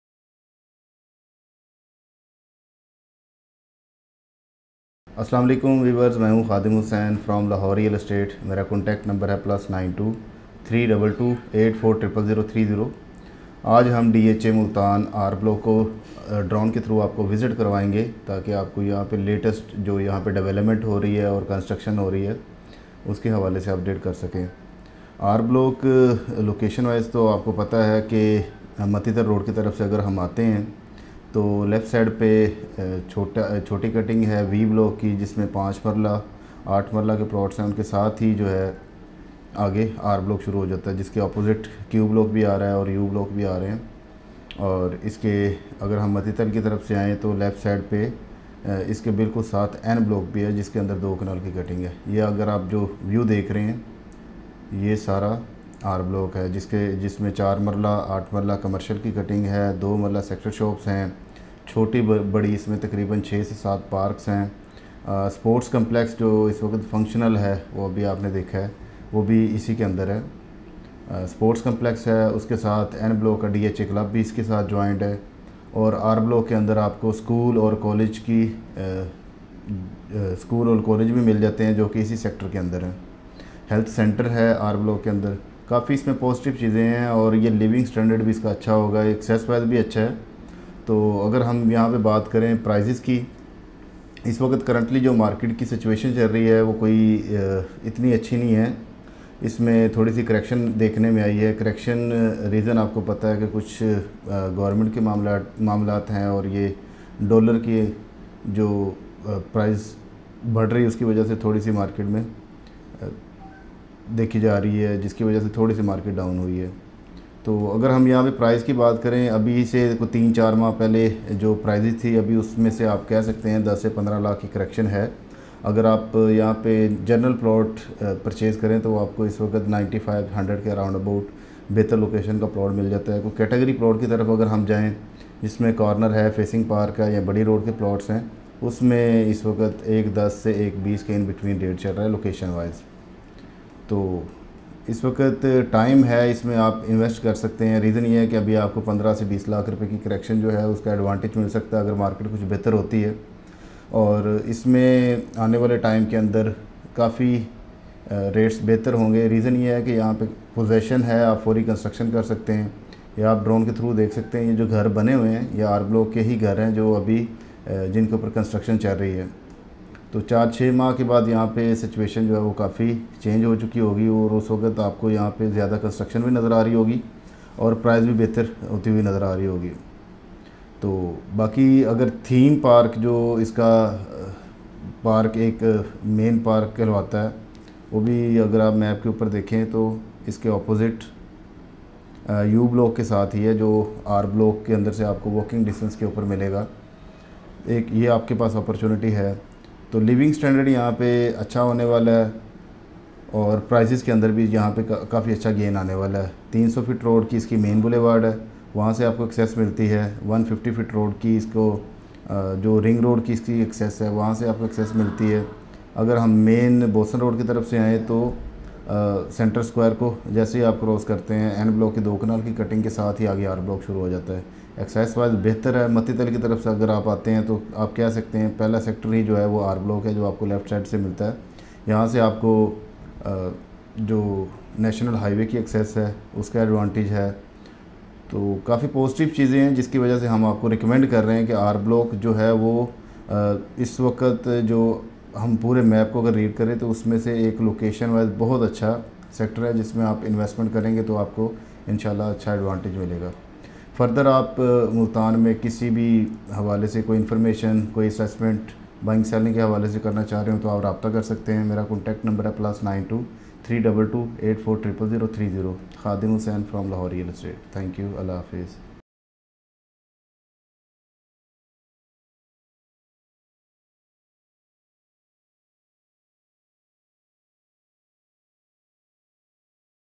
DHA Multan R Block | Latest Prices & Construction Overview | Drone Video | Jun 21, 2022
DHA Multan R Block | Latest Prices & Construction Overview | Drone Video | June 2022

✅ DHA Lahore
✅ DHA Bahawalpur
✅ DHA Gujranwala
✅ DHA Multan
✅ DHA Quetta

✅ DHA Peshawar
✅ Defence View Apartments Lahore
✅ Eighteen Islamabad